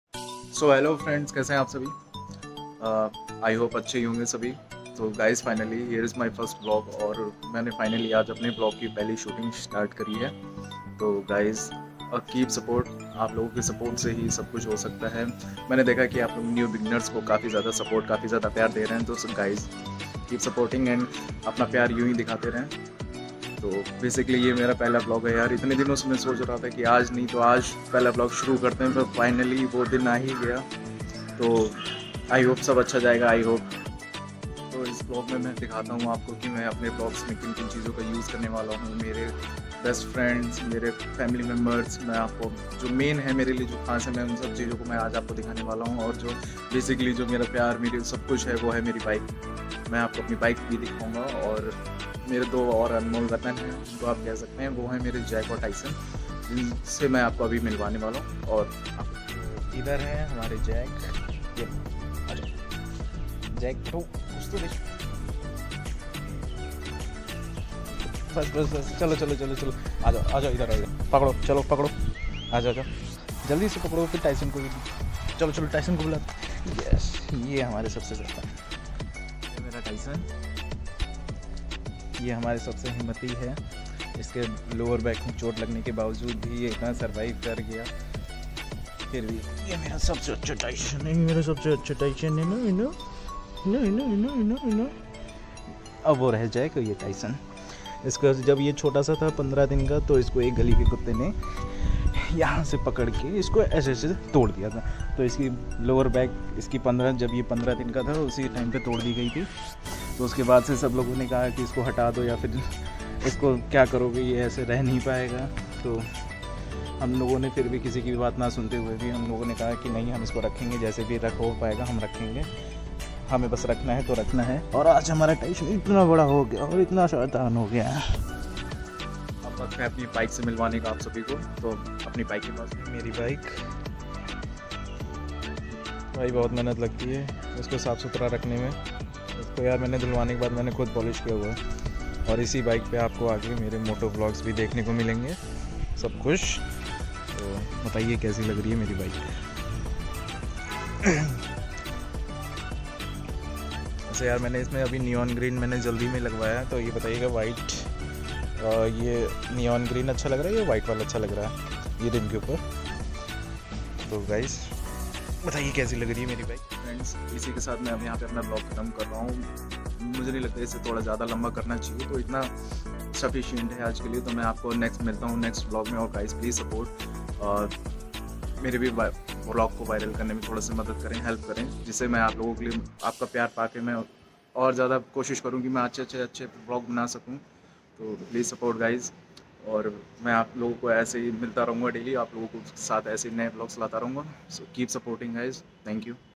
First vlog||🖤On the way to firstvlog
First vlog||🖤on the way to viral|
In this vlog i will introduce my bike and dog 🐕|
First vlog||
First Vlog
Youtube First Video
Kshitij Rajvanshi Vlogs